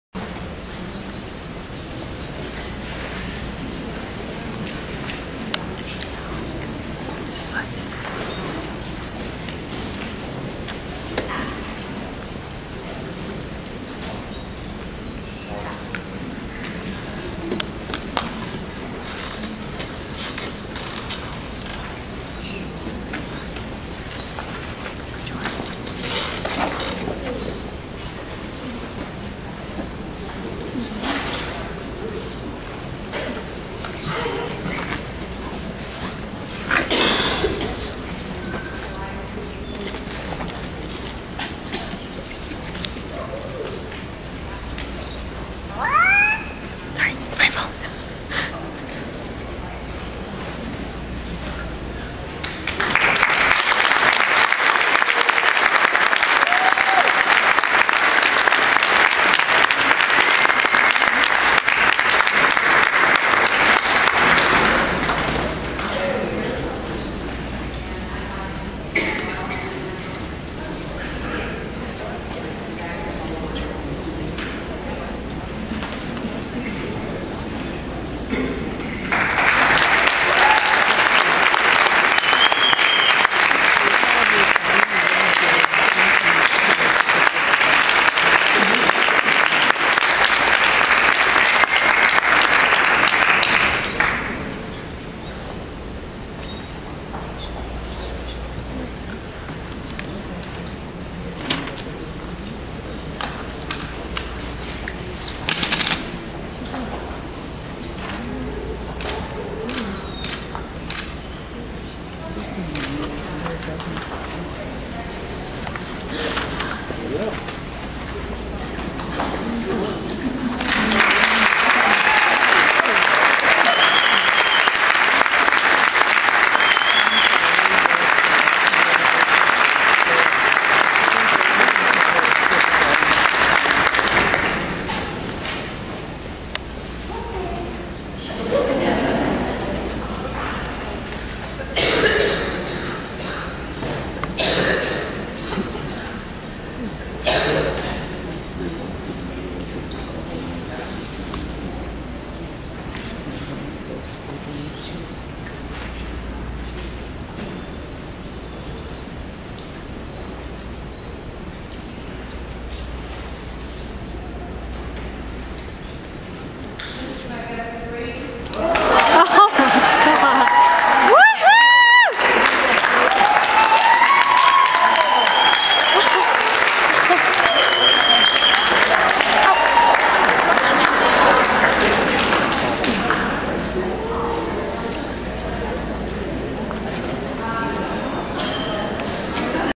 Dalmatian Club of America- Best of Breed 2012
May 9th, 2012. Tulsa OK DCA (Nationals) Best of Breed. Staring KRASH!  Judging by Beth White, Supporting co-star Jordan Cartwright and video by Debbie Sutor.